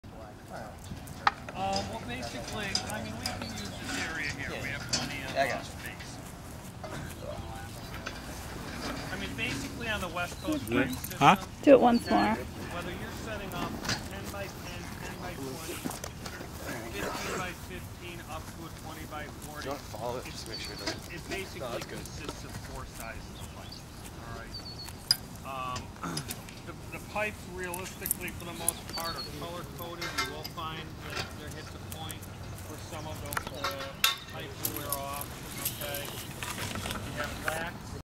Learn to Tie Knots to Install Pole Tents - Pt. 1 Attach Ropes To Stakes
Watch as our Tent Installation Expert teaches how to tie the perfect knot to safely attach your ropes to your tent stakes while installing pole tents